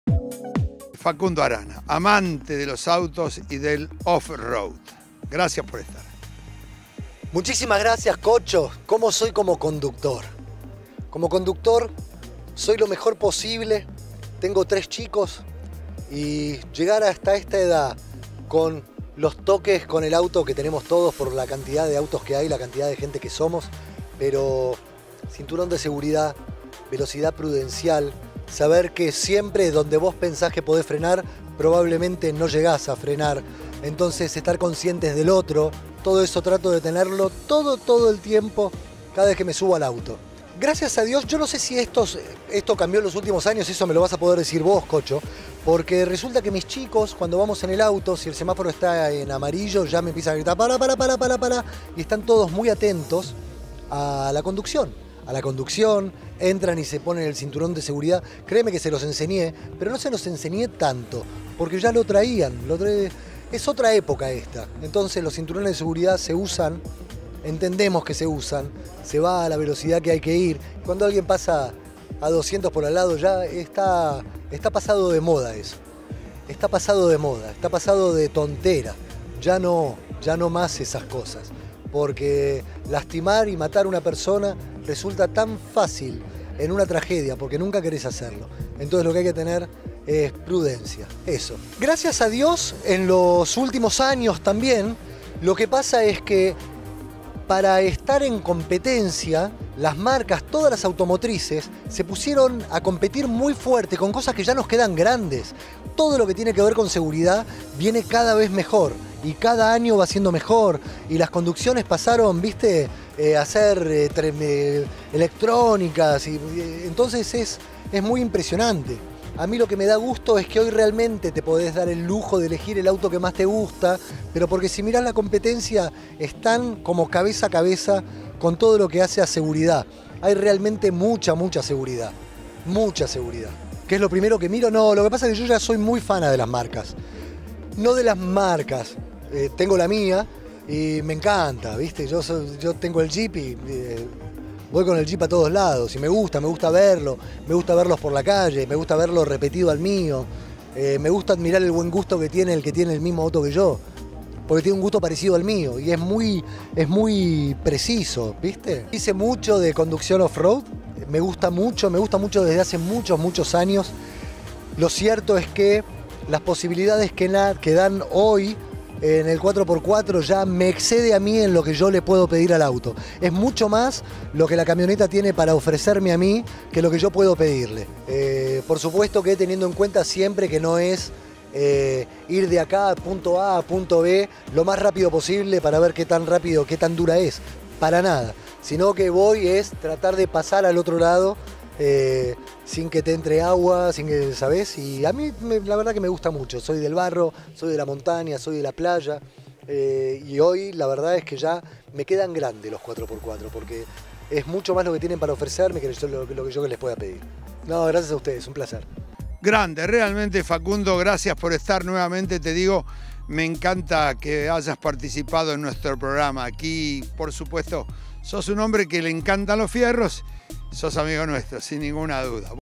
Facundo Arana "Ir a 200KM/H está pasado de moda"
Hablamos con el actor, que condujo la fiesta de premiación de los "Autos Más Seguros del año" de Cesvi Argentina sobre cómo es como conductor, su pasión por los autos y lo primero que le mira, entre otras cosas...